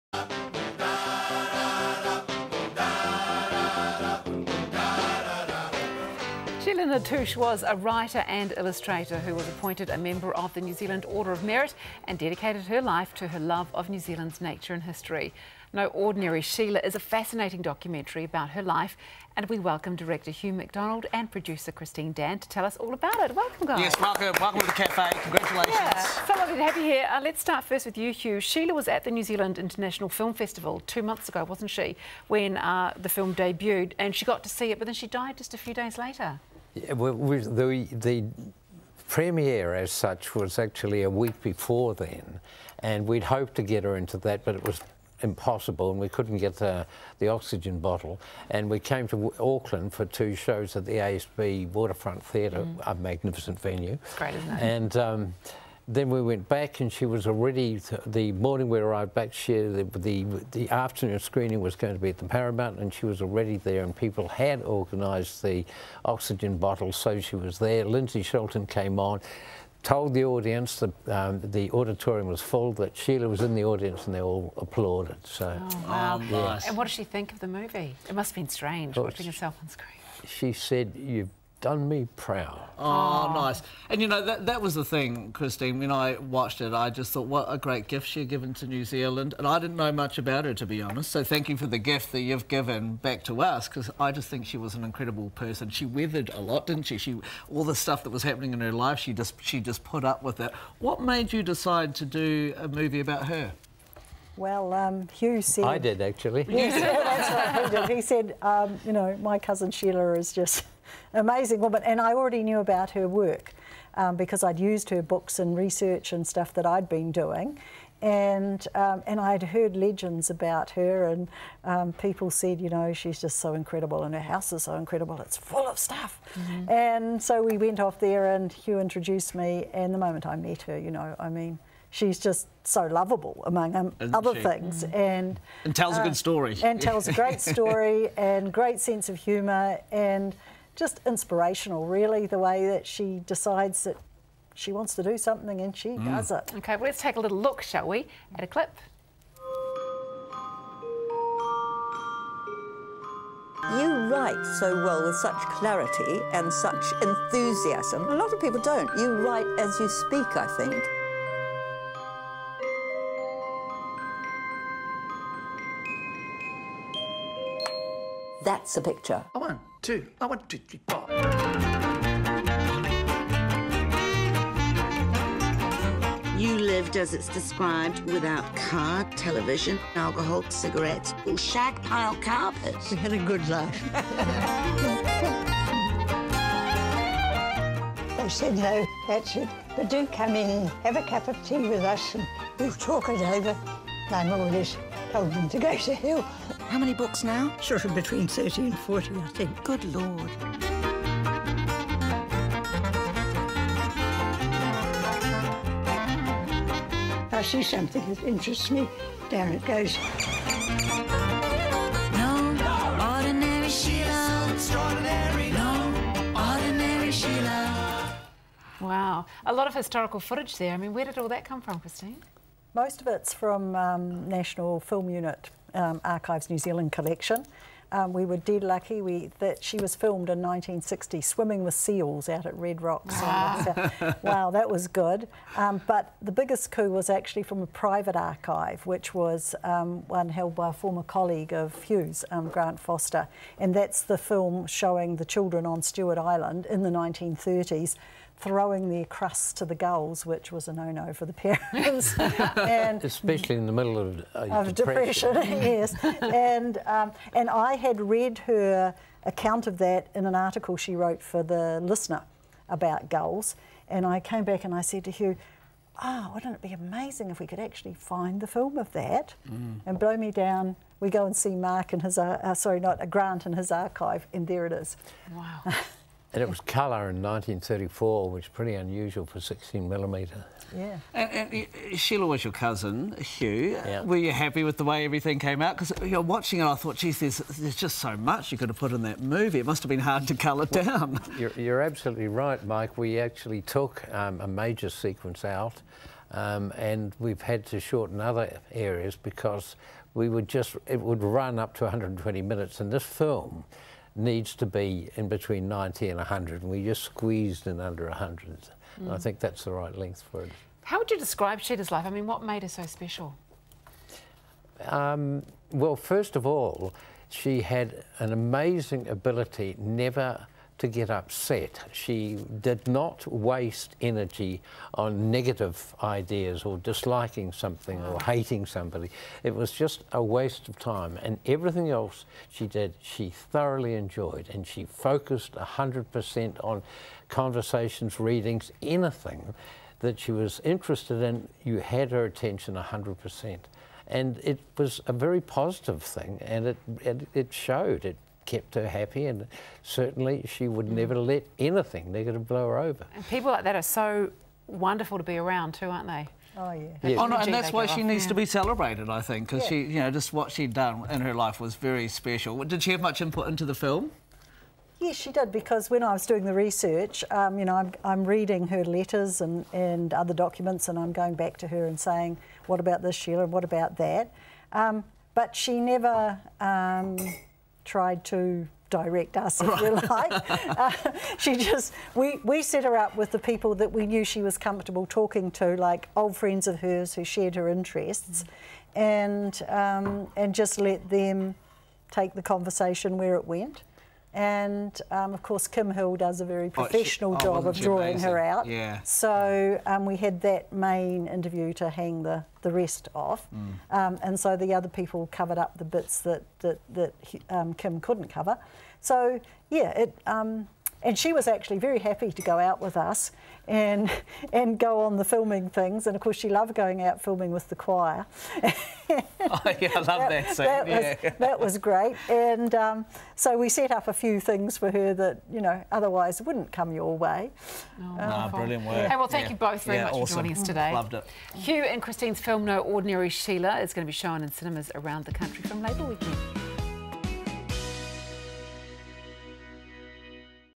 We chat to the creators of the ‘No Ordinary Sheila’ documentary
Sheila Natusch was a writer and illustrator who was appointed a Member of the New Zealand Order of Merit and dedicated her life to her love of New Zealand’s nature and history. ‘No Ordinary Sheila’ is a fascinating documentary about her life and this morning the director Hugh Macdonald and producer Christine Dann joined us to tell us all about it.